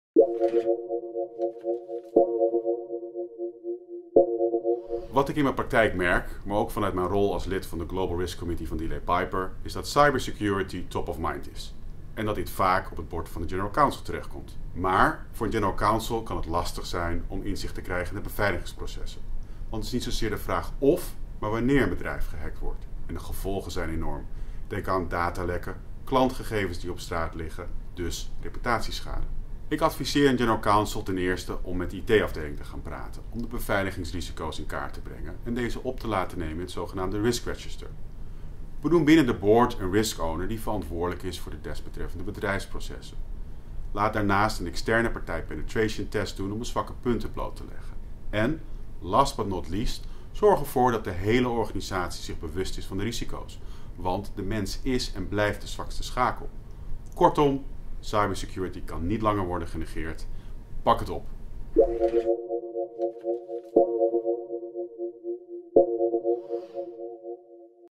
Sixty seconds - Cyber security: vier tips voor de general counsel
Videoblog Sixty Seconds door Joris Willems van DLA Piper. In deze vlog legt hij uit welke stappen een general counsel moet nemen rondom het thema cyber security.